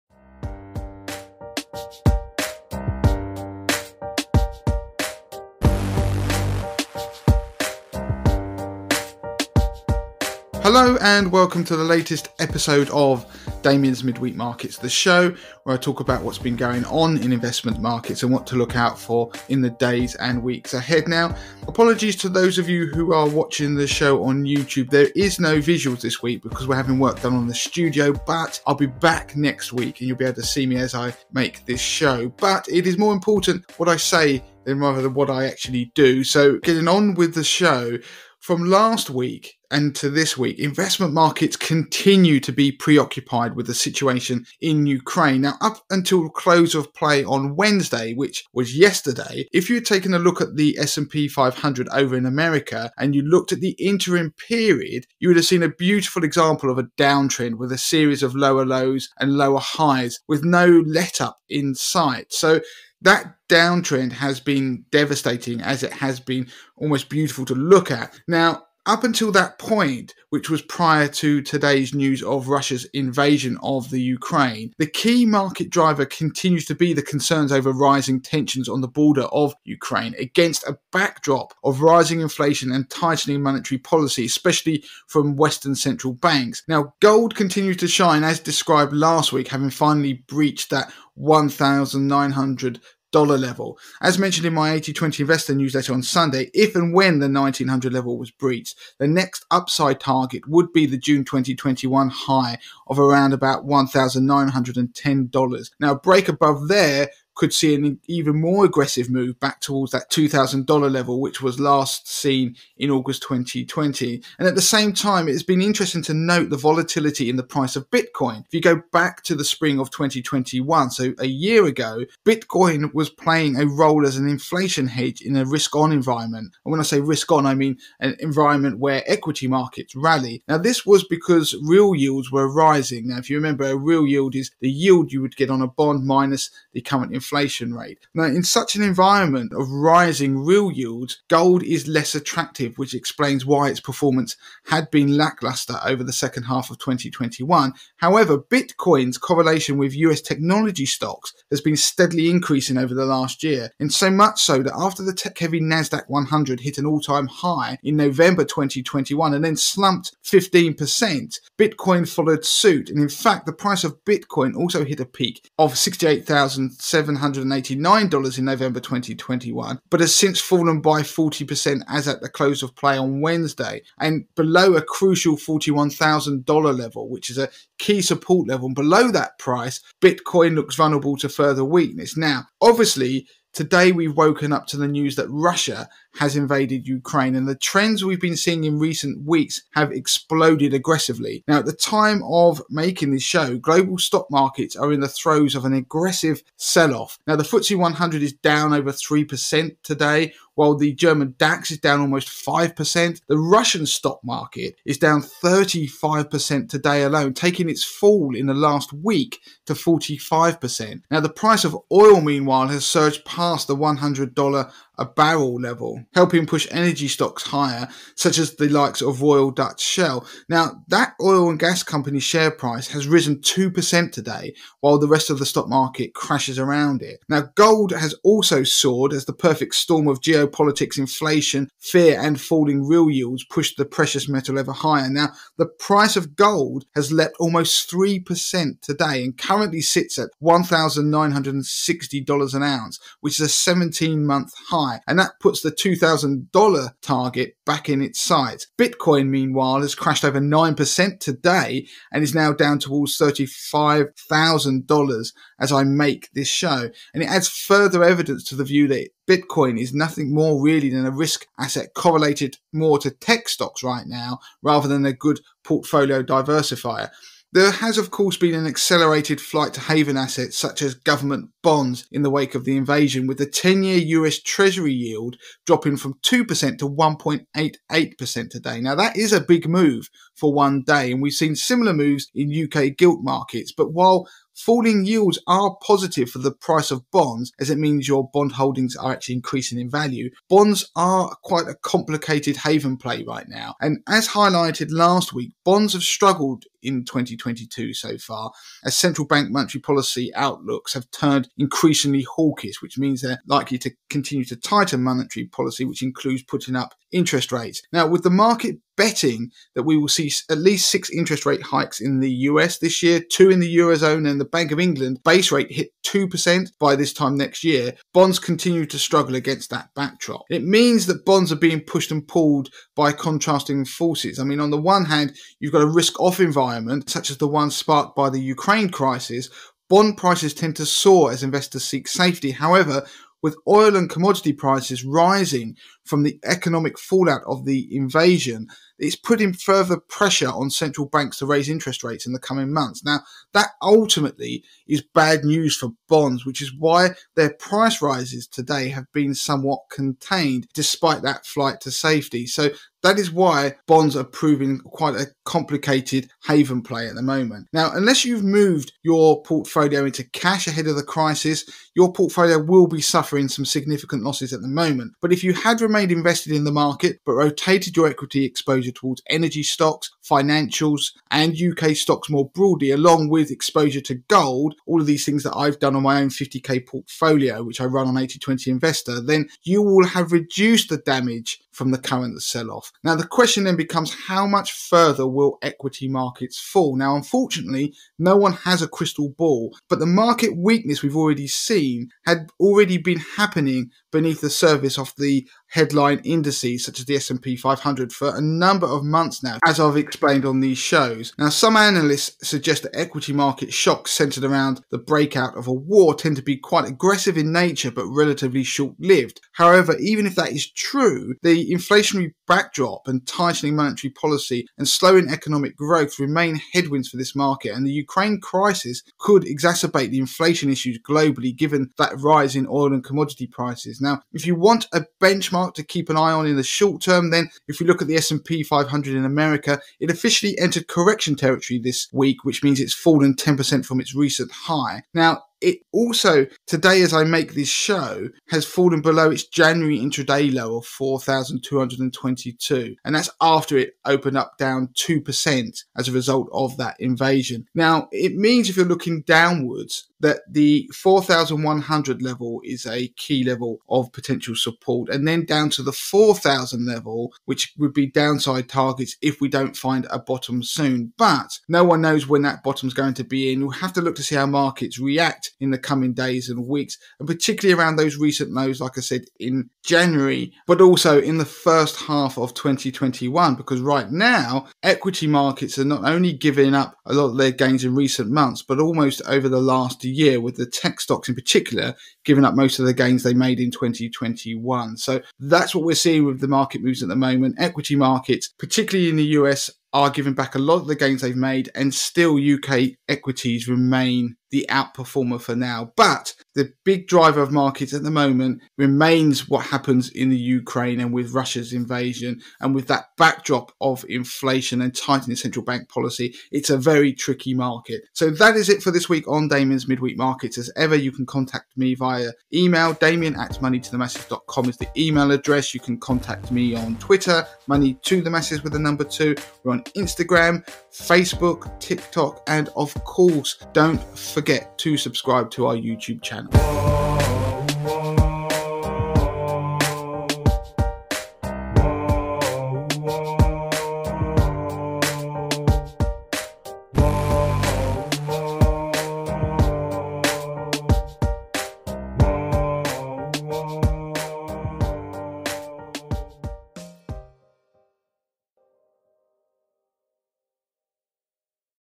Markets react after Ukraine invasion | Damien's Midweek Markets
This week I talk about the impact of the Ukraine invasion on investment markets.

My name is Damien Fahy, founder of and each week I discuss what is happening in investment markets so that DIY and novice investors can make sense of what is affecting their investments.

If you are looking to get your finances in shape then check out Damien's FREE Money MOT